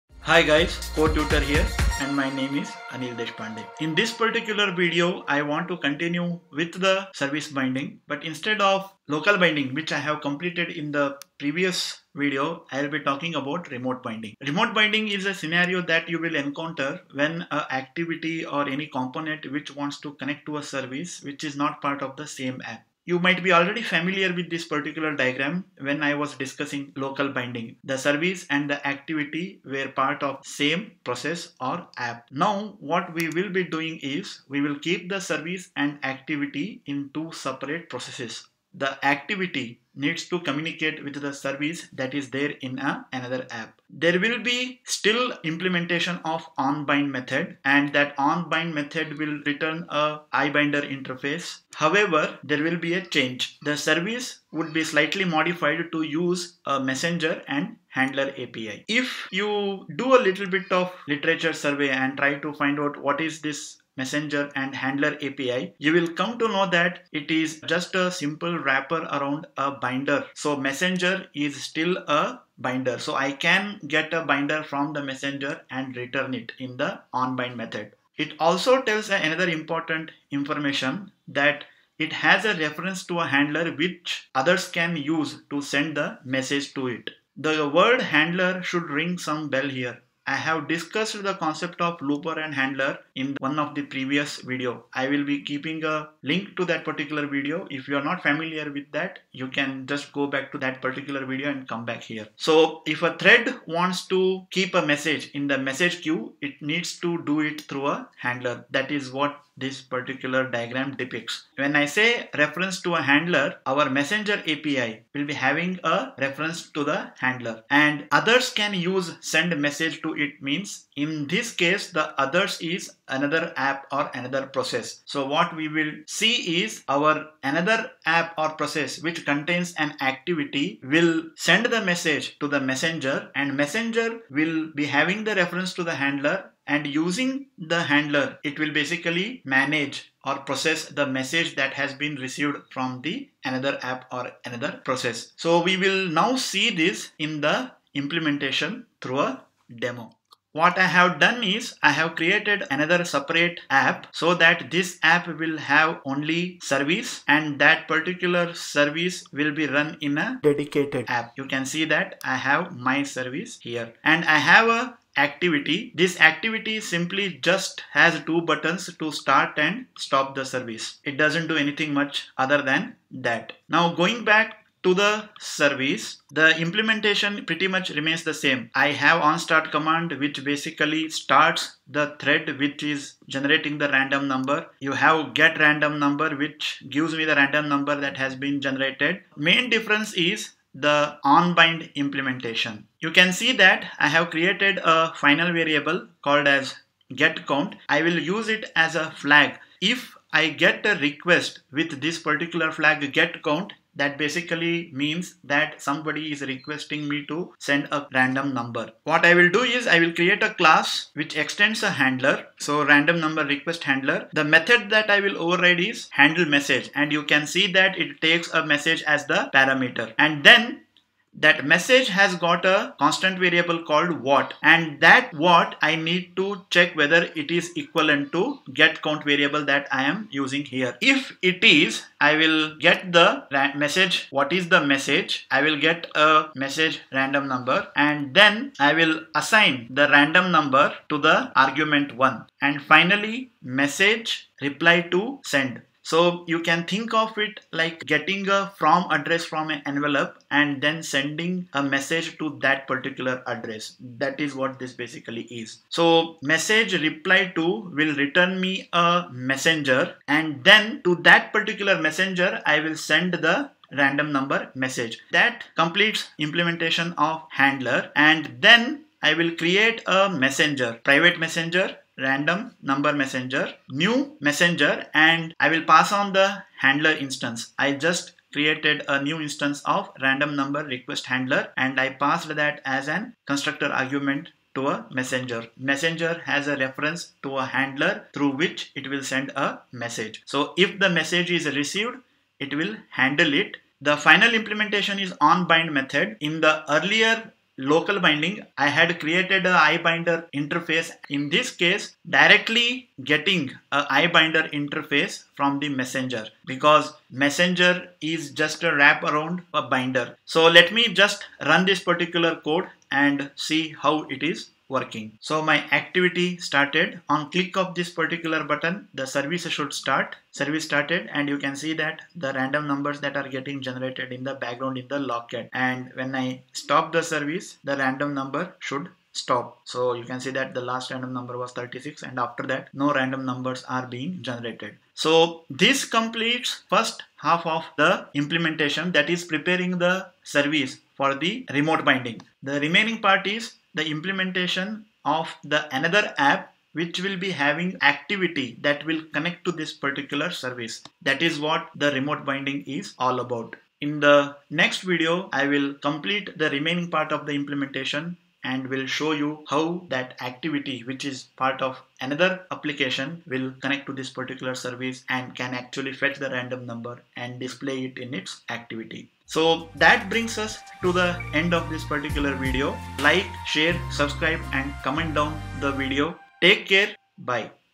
Services in Android - Part 6, Remote binding Service using Messenger
This video gives a basic introduction of Messenger API and explains how it is used to prepare the Service to handle the messages from the remote process/app. It also demonstrates the implementation of Handler API which is used by the Messenger to handle the incoming messages.